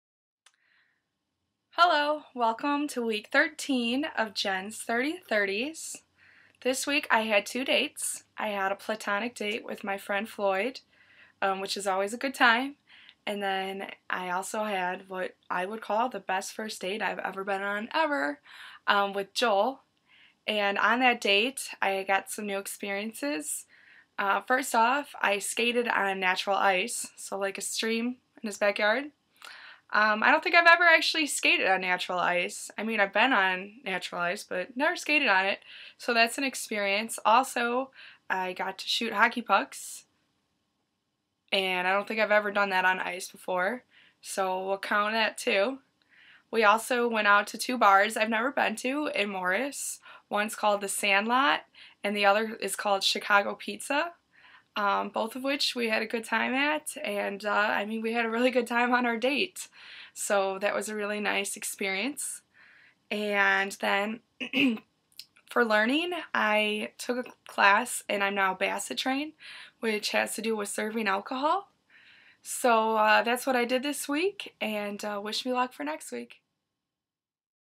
Week 13 Date and Skate
Date with Floyd and Joel, Skated on real ice, Shot hockey pucks, Sand Lot, Chicago Pizza, Basset